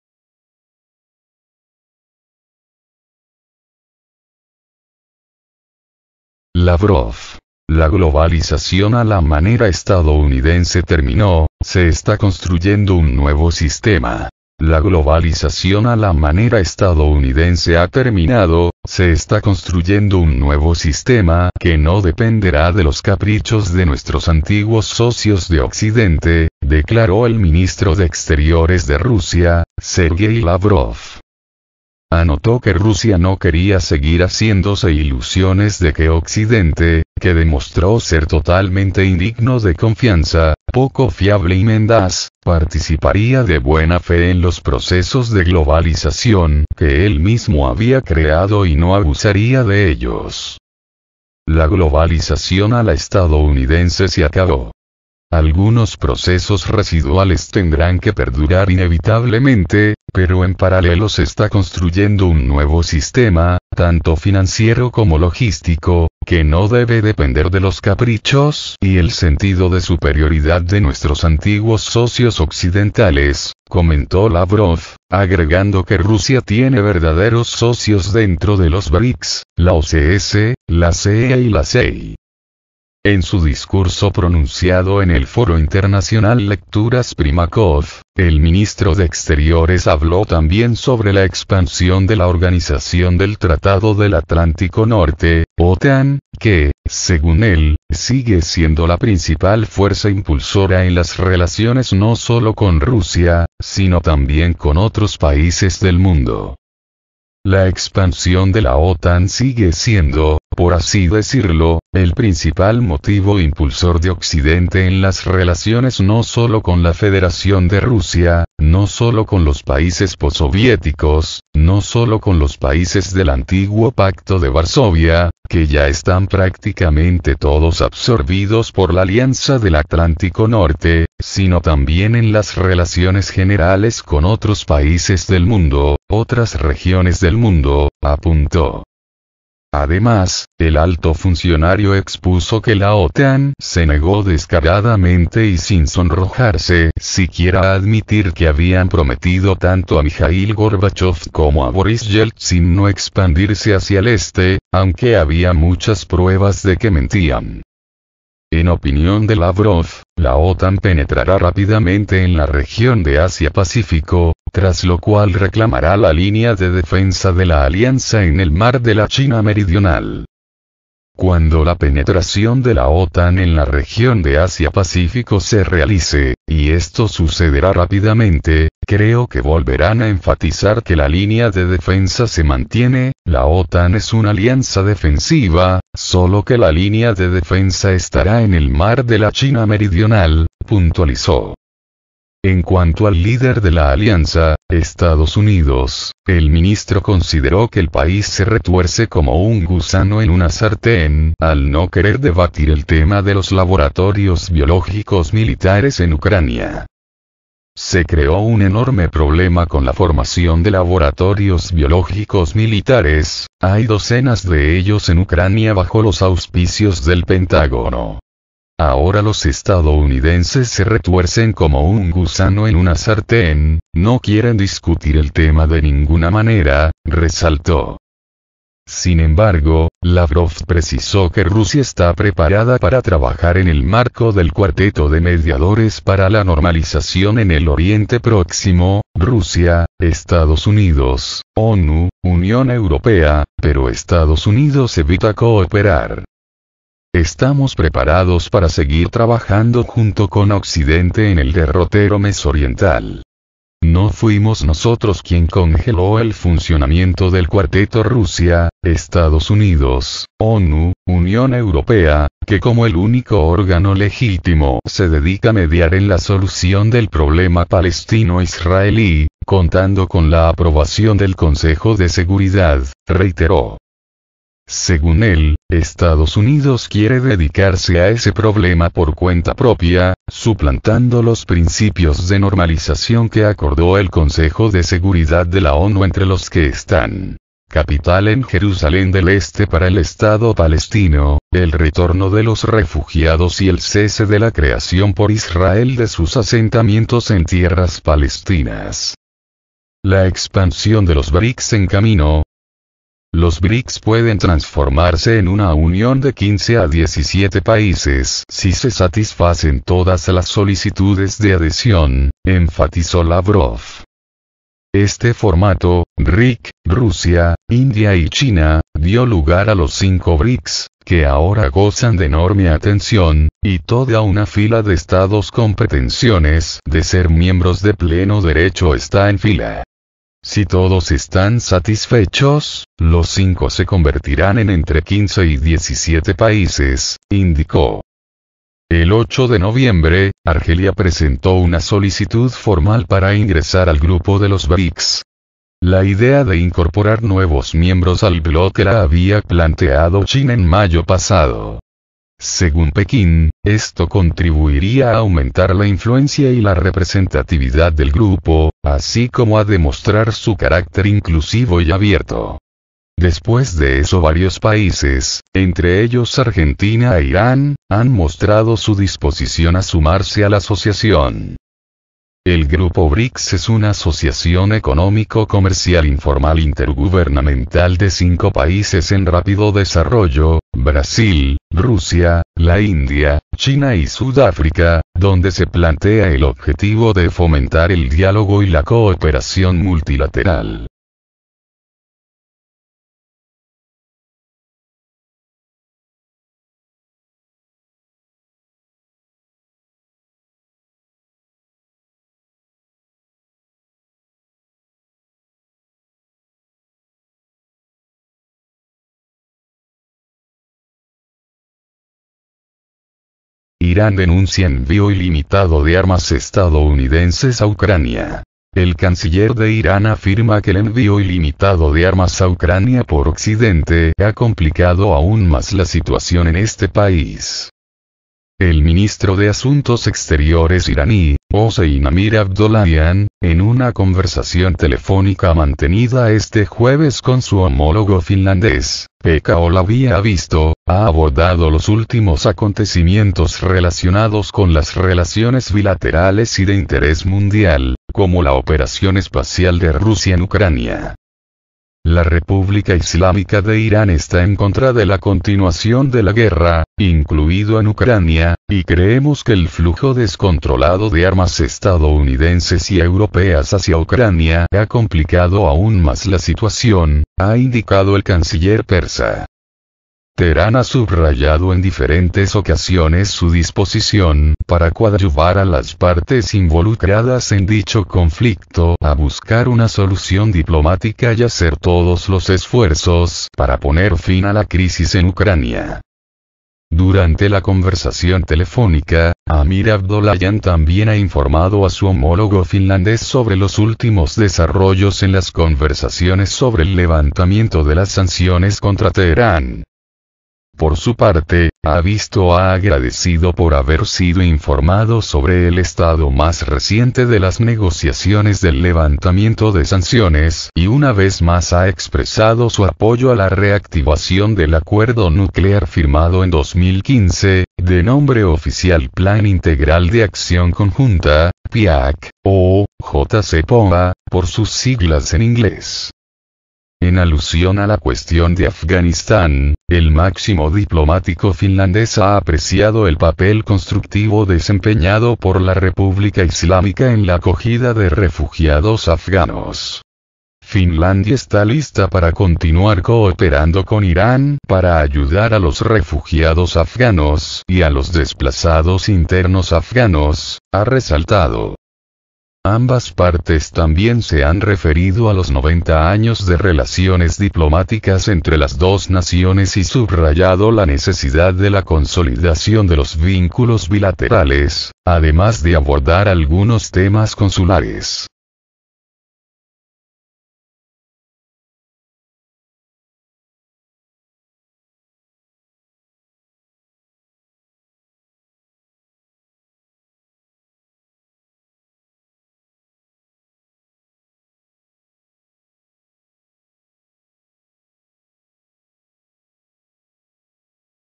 Rusia: La globalización a la manera estadounidense terminó, se construye un nuevo sistema más justo
Irán denuncia envío ilimitado de armas estadounidenses a Ucrania